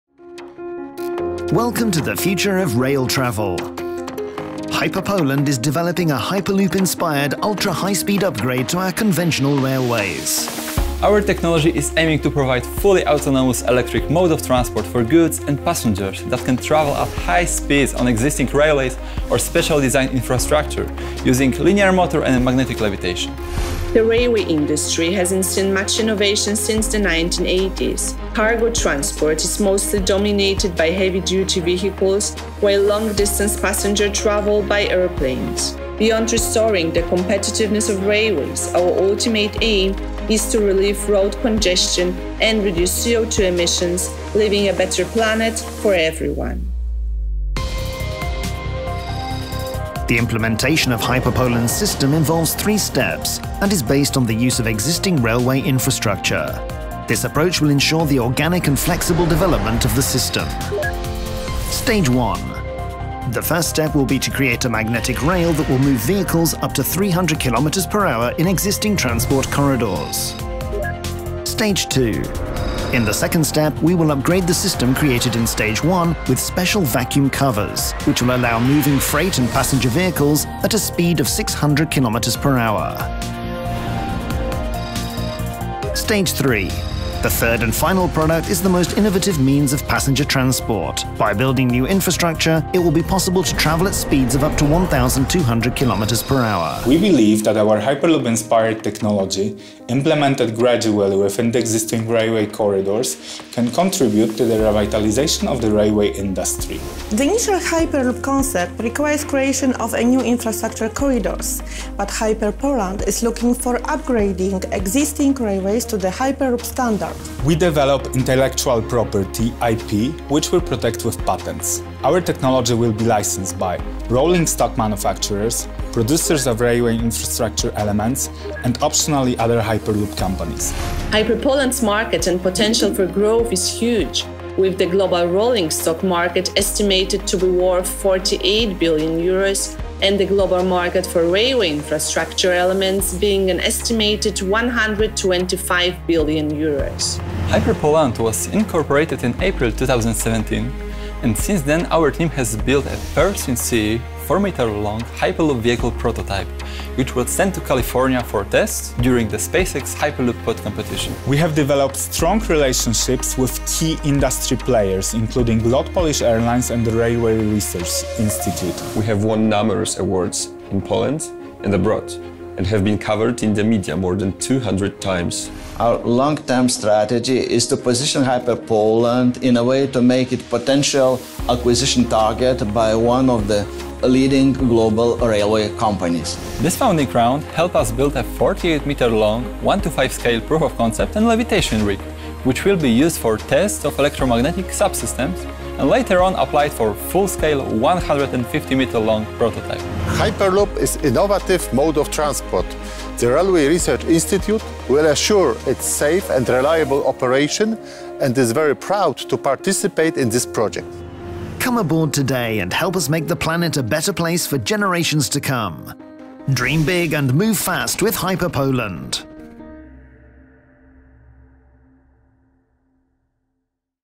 Join the hyperloop movement | Nevomo
Nevomo is developing a Hyperloop-inspired ultra-high speed upgrade to conventional railways. At first, we can operate on existing railway tracks up to a max. speed: 550 kph (while keeping the functionality of conventional railways). Then we can upgrade it into a vacuum system (up to 600 kph/370 mph on existing tracks and 1,200 kph/745 mph on new routes).